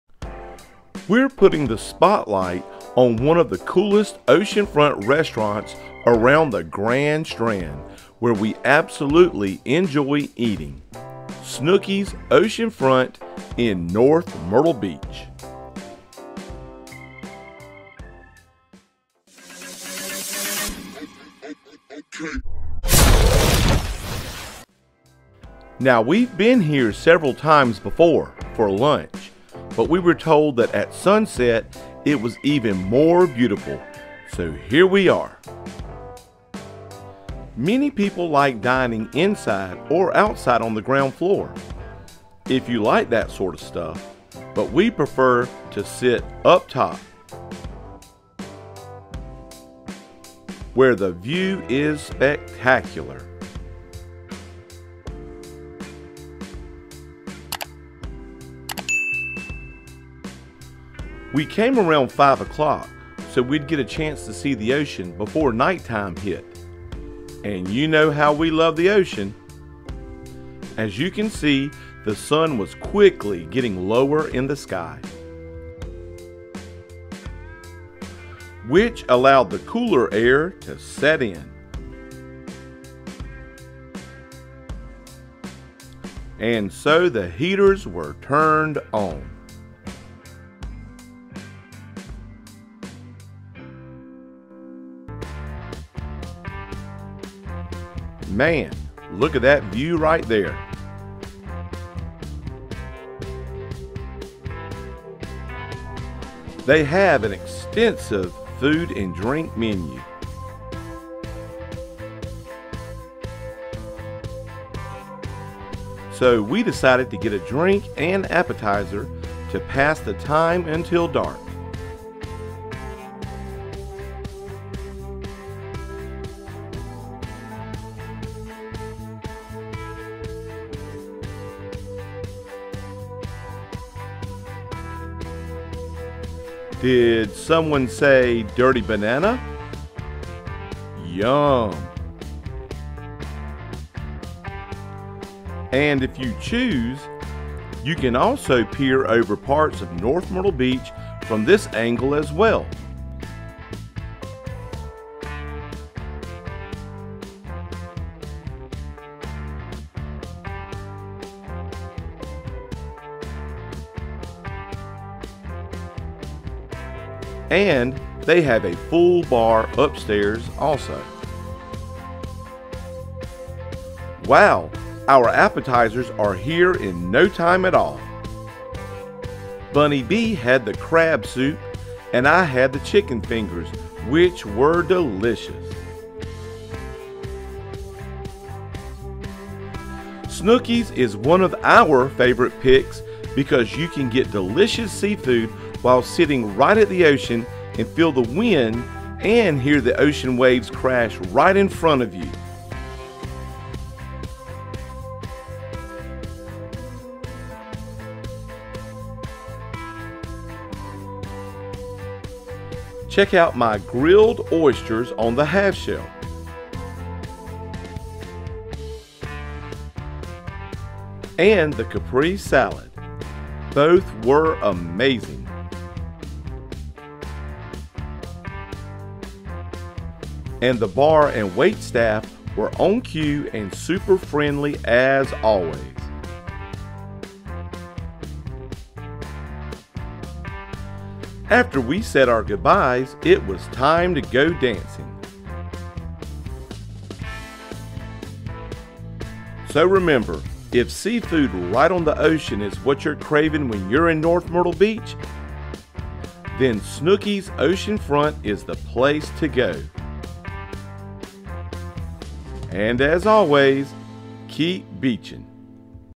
SNOOKY’S Ocean Front. Is where to eat in NORTH MYRTLE BEACH – Overview | 2021.
It’s no wonder why so many people go to this ocean front establishment. It is one of the coolest places around. And the view from the top deck is amazing.